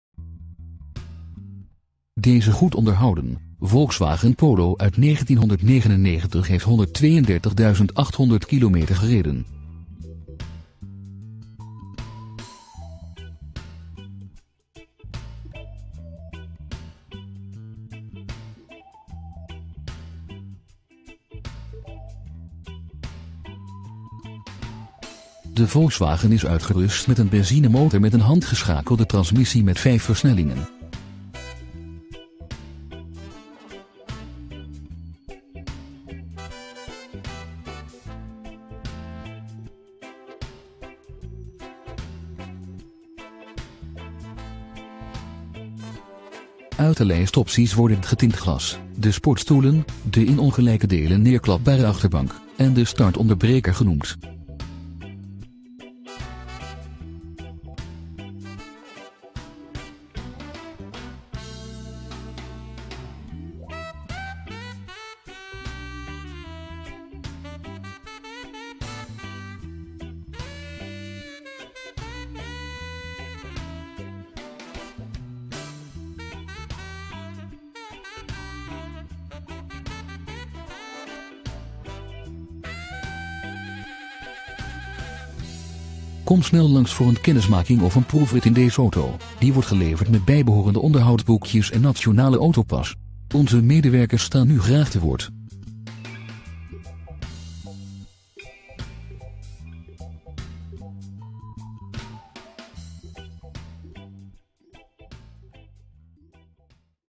Volkswagen Polo 1.4 Atlanta 132dkm APK t/m 02-2016 Inruil mogelijk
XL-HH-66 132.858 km
  Prijs:     € 1.248
  Merk:     Volkswagen
  Model:     Polo
  Type:     1.4 Atlanta 132dkm APK t/m 02-2016 Inruil mogelijk
  Bouwjaar:     1999
  Transmissie:     handgeschakeld
  Kleur:     Blauw
  BTW/Marge:     marge
  Brandstof:     benzine
  Energielabel:

Accessoires
* 3e remlicht
* Achterruitwisser(s) met sproeier
* Airbag bestuurder
* Airbag passagier
* Getint glas
* Radiovoorbereiding
* Ruitenwisser achter
* Sportstoelen
* Startonderbreker

Boekjes: Aanwezig

Een nette, goed onderhouden auto. APK t/m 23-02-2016. De distributieriem is vervangen bij 89965 kilometer.
7825 XE EMMEN (rondweg)